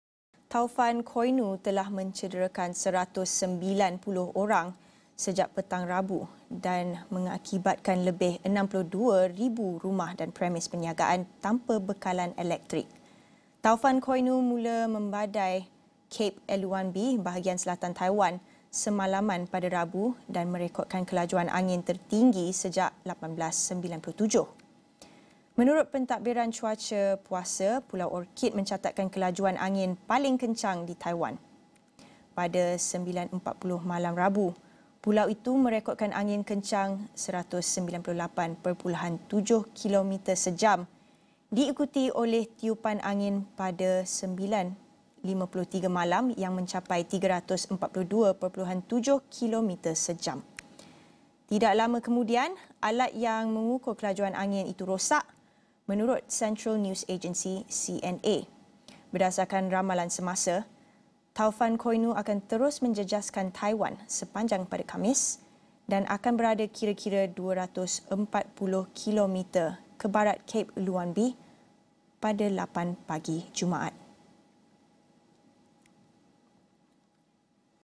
Taufan Koinu catat kelajuan tertinggi sejak 1897
Taufan Koinu telah mencederakan 190 orang sejak petang Rabu dan mengakibatkan lebih 62,381 rumah dan premis perniagaan tanpa bekalan elektrik.

Taufan Koinu mula membadai Cape Eluanbi, bahagian selatan Taiwan semalaman pada Rabu dan merekodkan kelajuan angin tertinggi sejak 1897.